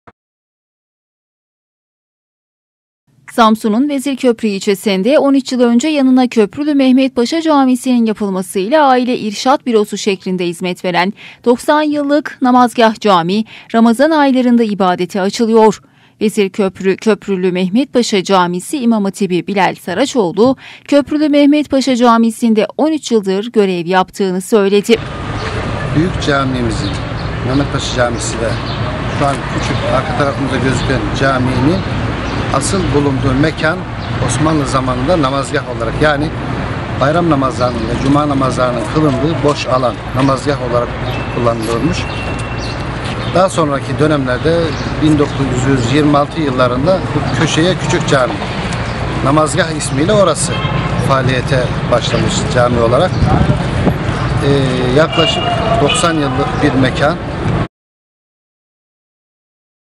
90 YILLIK NAMAZGAH CAMİ CEMAATİNE KAVUŞUYOR
Samsun'un Vezirköprü ilçesinde, 13 yıl önce yanında Köprülü Mehmet Paşa Camisi'nin yapılmasıyla "aile irşad bürosu" olarak hizmet vermeye başlayan.